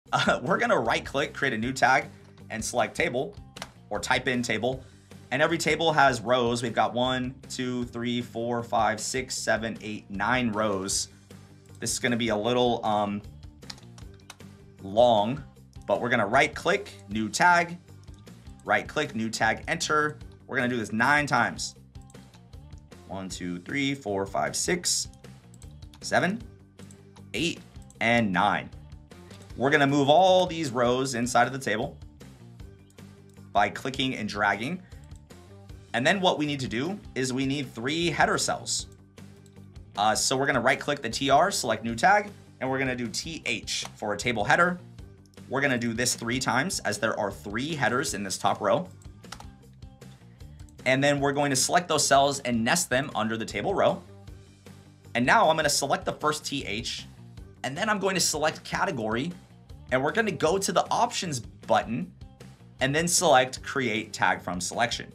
Add Tags from Scratch in Adobe Acrobat Pro DC | PDF Accessibility
This step-by-step tutorial shows how to build table, row, header, and cell tags from scratch to make PDFs fully accessible.